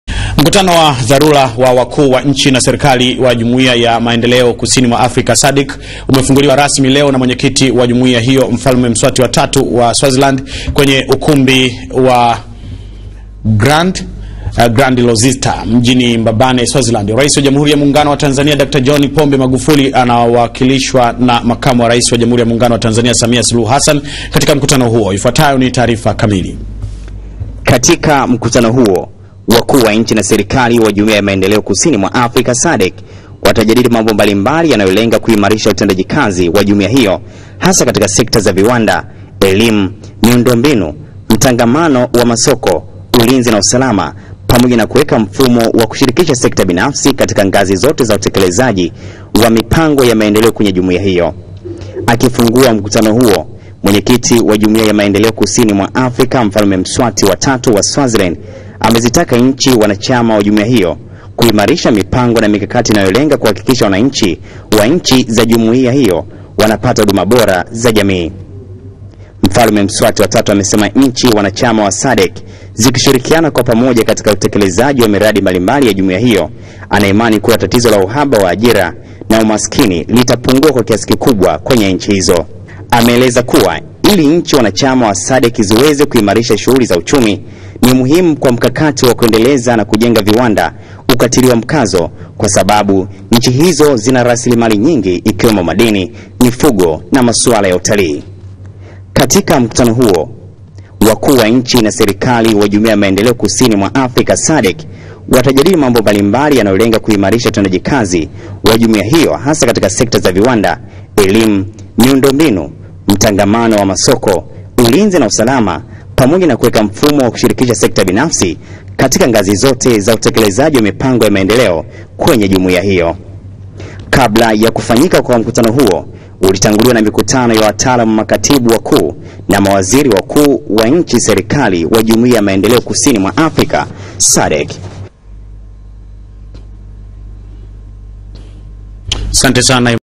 Sadc Waitisha Mkutano Wa Dharula
Mfalme Muswati wa Swaziland amefungua mkutano wa dharula wa SADC kwa ajili ya kujadili utendaji kazi wa mambo mbalimbali ya jumuiya hiyo.